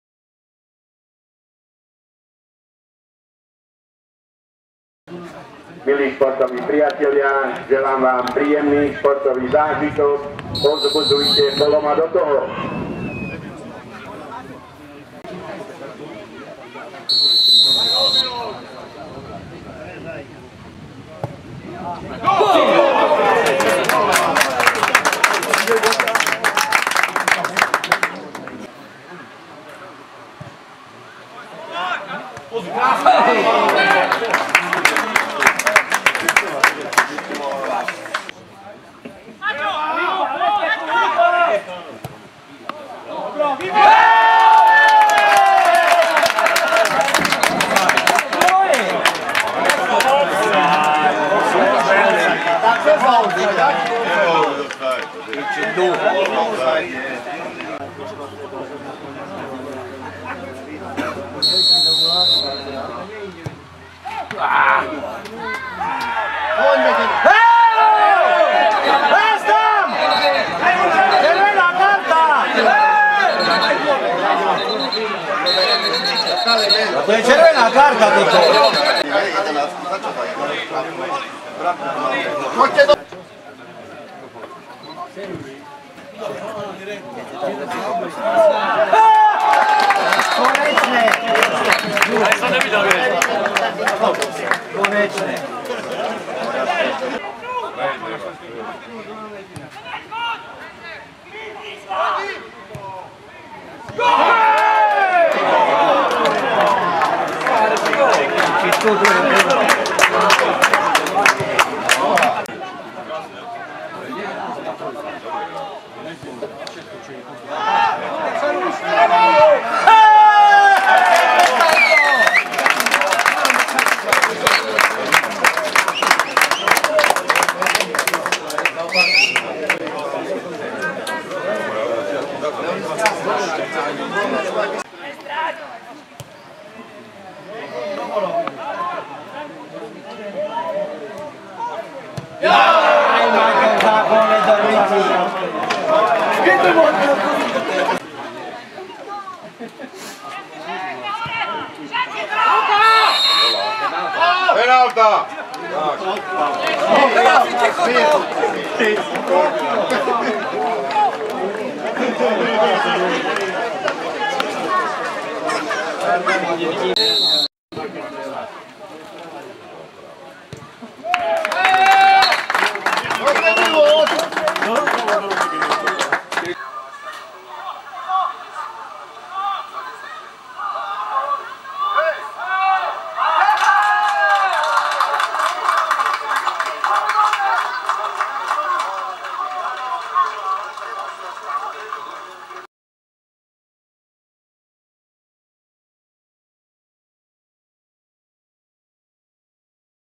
Gem. Poloma - Krh. Podhradie 3:1 (2:0)
Benedik (26), Macák (34), Mil. Gallo ( 55 z 11 m) - R. Kuzma (61), ČK: Harhovský (18)